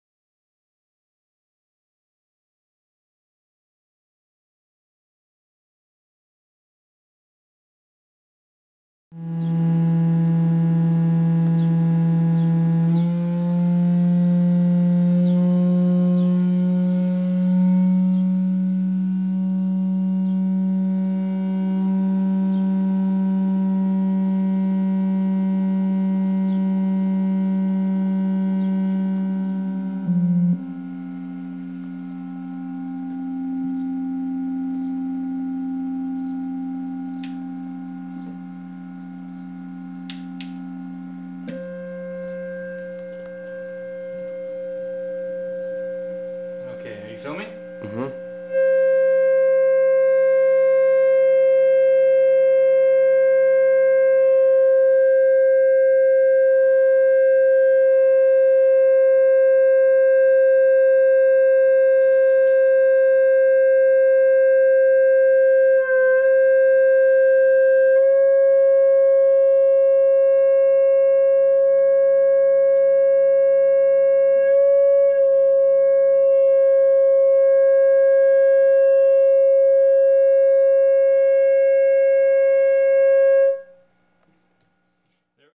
Chladni Plate Patterns
Some older footage from the 90s of me using Chladni Plate frequency/pattern techniques to help tune the top and back of an acoustic guitar.